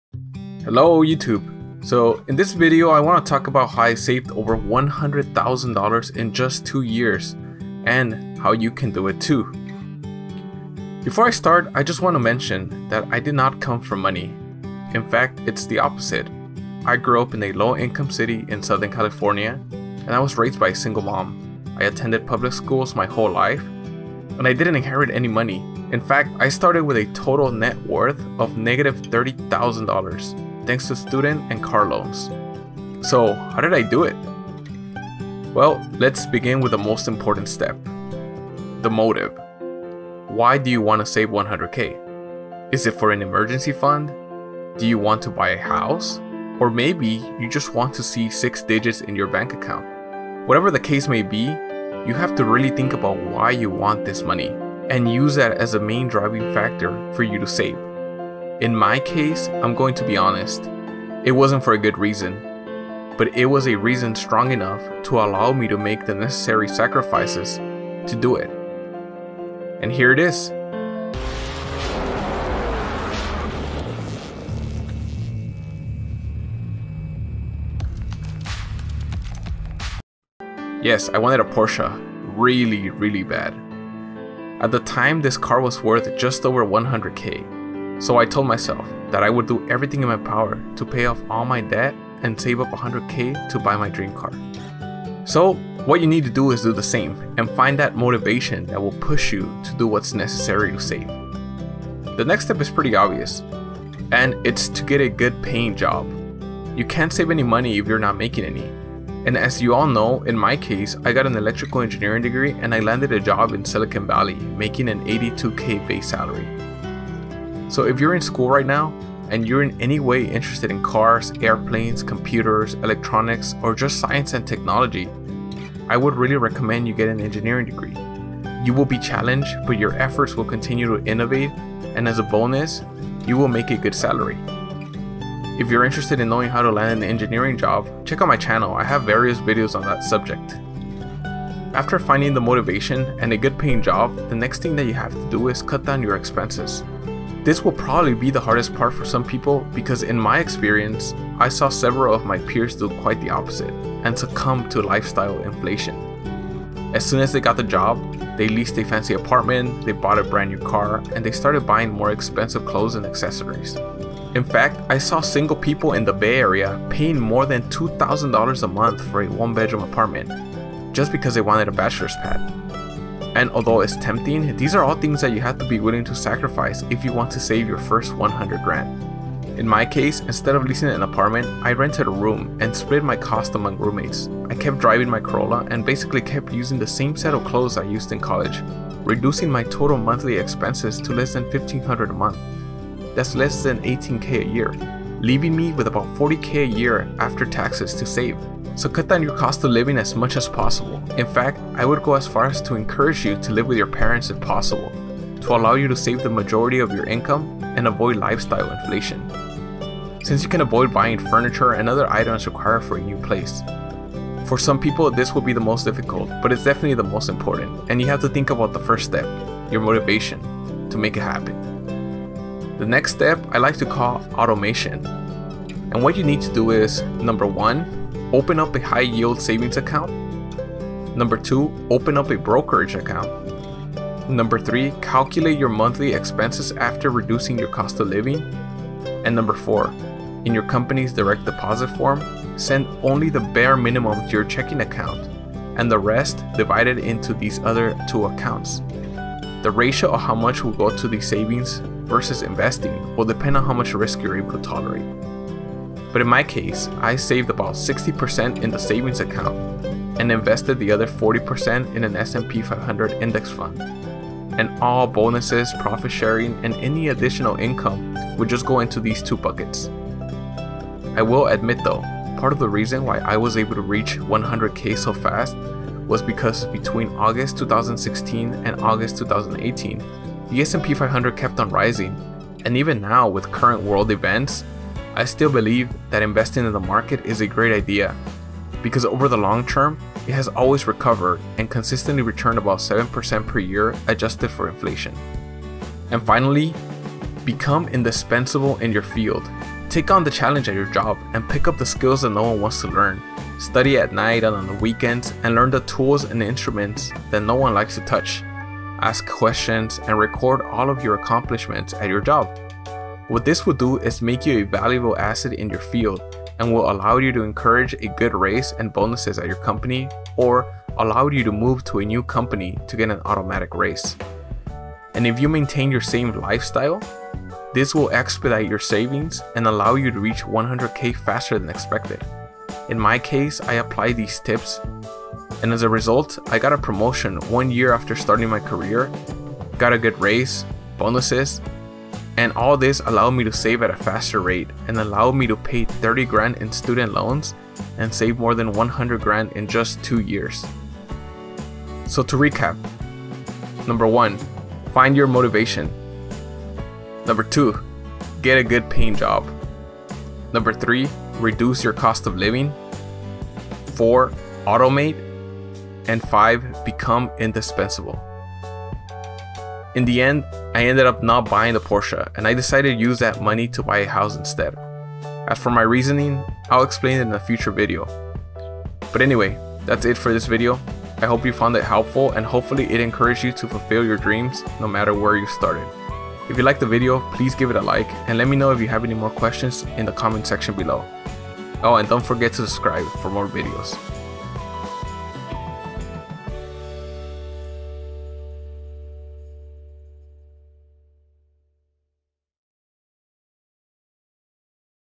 Saving $100,000 by age 26(as an Engineer)
In this video I want to talk about how I saved over 100 thousands dollars in just two years and how you can do it too!

Before I start I just want to mention that I did not come from money, in fact it’s the opposite, I grew up in a low income city in southern California, I was raised by a single mom, I attended public schools my whole life. I didn’t inherit any money and I started with a net worth of negative $30,000 thanks to student and car loans. So how did I do it?

Well, let’s begin with the most important step, the motive, why do you want to save 100k? Is it for an emergency fund? Do you want to buy a house? Or maybe you just want to see 6 digits in your bank account? Whatever the case may be, you have to really think about why you want this money? And use that as the main driving factor for you to save.  In my case and I’m going to be honest, it wasn’t for a good reason but it was a reason strong enough to allow me to make the necessary sacrifices to do it.